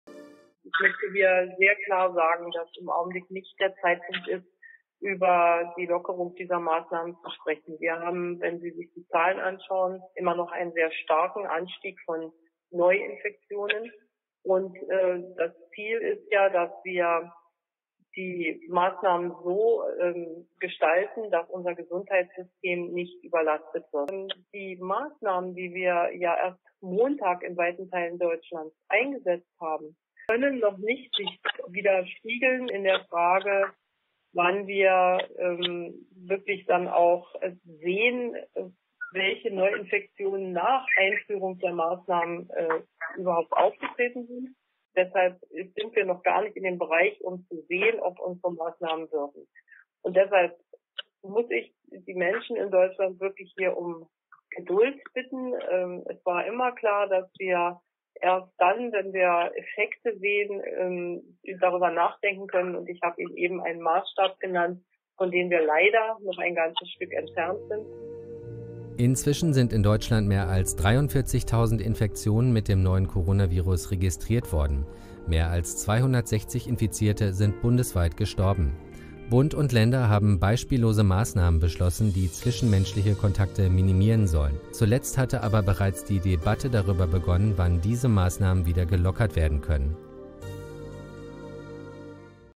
Merkel: debate on relaxing corona measures too early
The Chancellor has expressed her opinion on a relaxation of the corona measures. Click here for more current news about the corona crisis in Germany